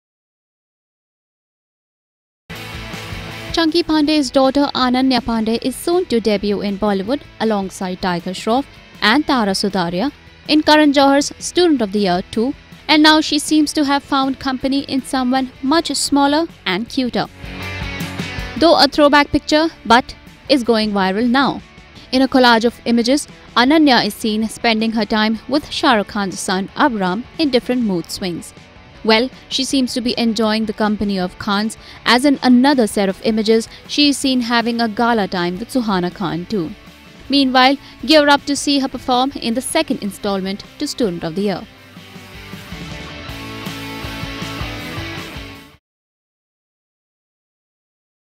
Karan's 'student' Ananya finds company in SRK's son AbRam
Chunkey Pandey's daughter Ananya Pandey is soon to debut in Bollywood alongside Tiger Shroff and Tara Sutaria in Karan Johar's 'Student of the Year 2' and now she seems to have found company in someone much smaller and cuter. Meanwhile, gear up to see her perform in the second instalment to 'Student of the year'.